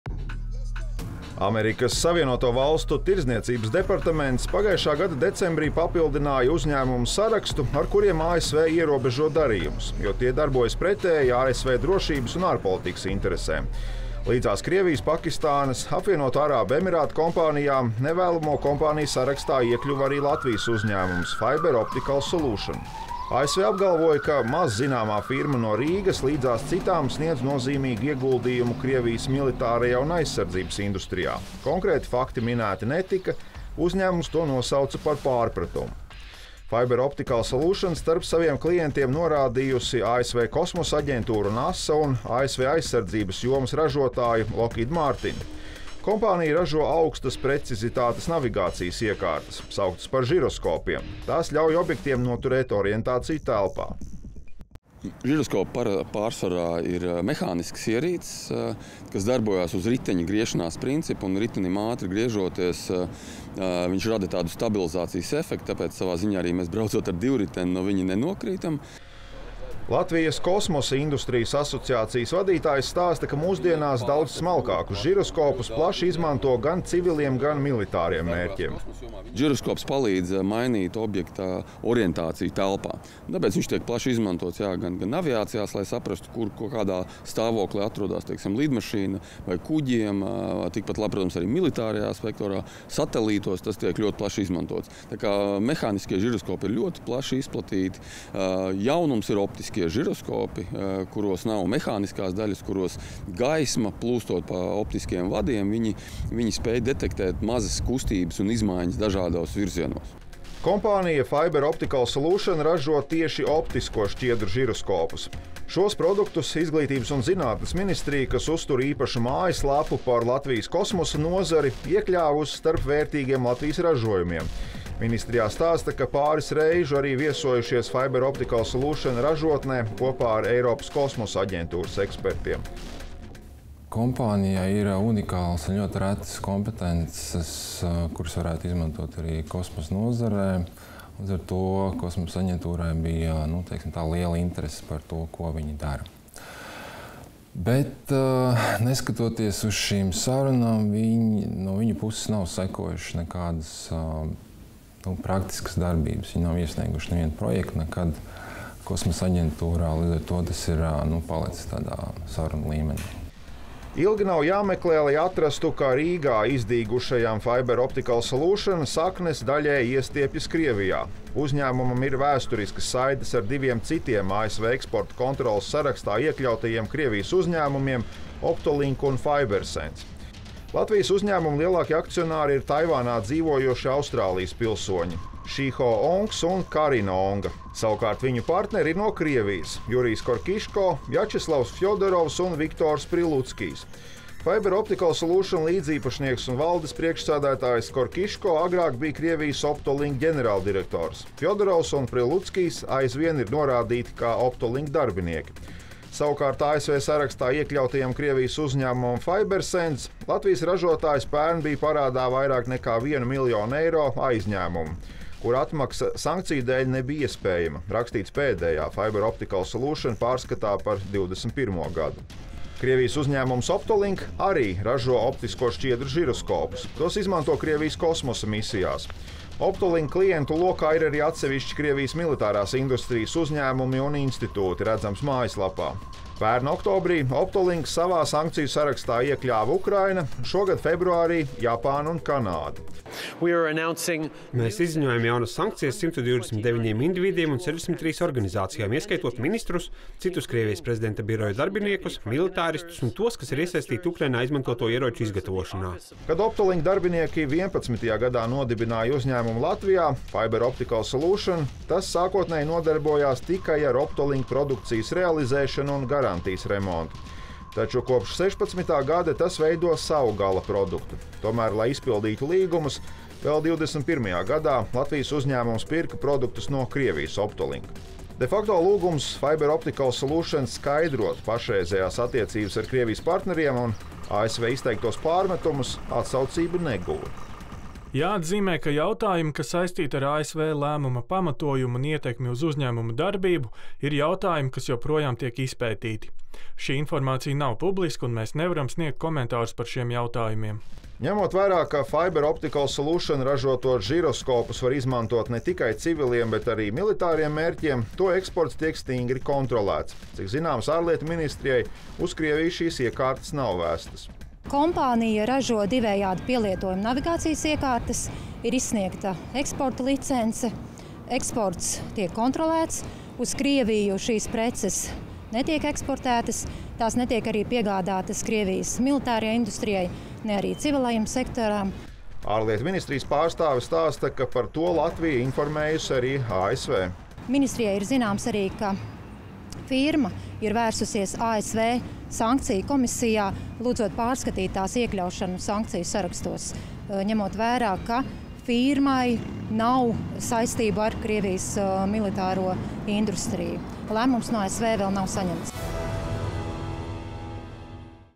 Kas ir ražotājs no Latvijas ASV nevēlamo firmu sarakstā?
Pagājušā gada decembrī Amerikas Savienotās valstis pavēstīja, ka viens no Latvijas uzņēmumiem palīdzējis Krievijas militārajai industrijai. Runa ir par uzņēmumu “Fiber Optical Solution”, kas ražo modernas navigācijas iekārtas. Uzņēmumu iekļāva tā sauktajā eksporta kontroles sarakstā. ASV lēmuma iemesli nav zināmi, bet noskaidrojām, ka šis Latvijas uzņēmums vēsturiski ir saistīts ar divām Krievijas kompānijām. Latvijas Ārlietu ministrijai nav apstiprinājuma, ka Latvijas ražotājs būtu apgādājis Krievijas militāro industriju. Un pats uzņēmums iekļaušanu sarakstā ir pārsūdzējis.